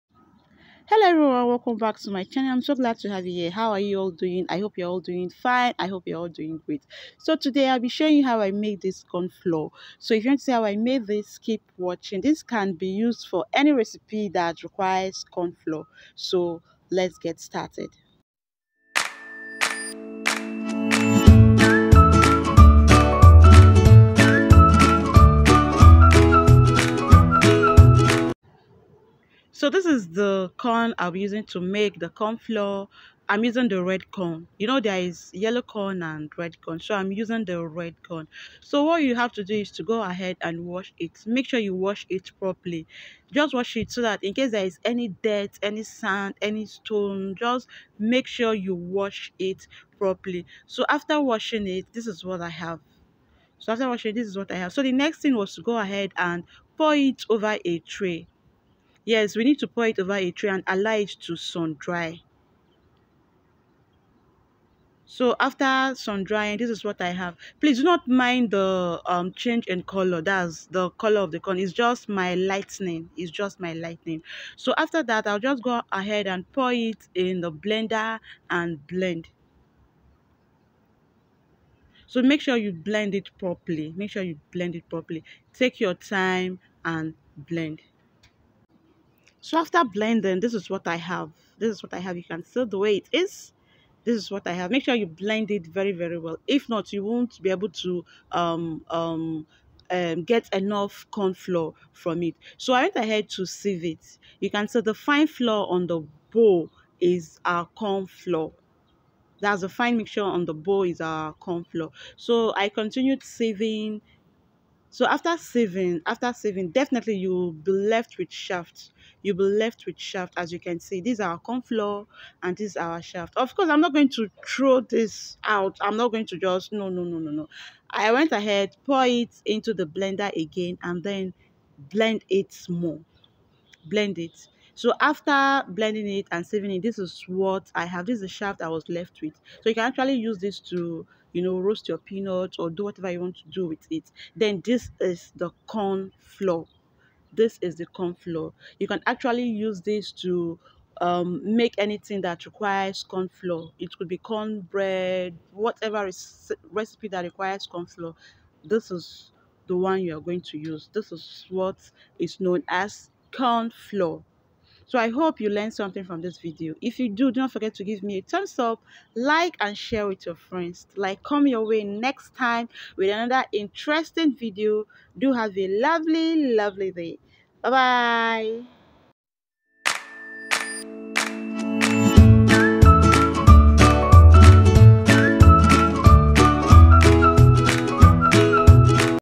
HOW TO MAKE CORN FLOUR | STELLA NDUKAIFE
Here's a detailed video on how to make corn flour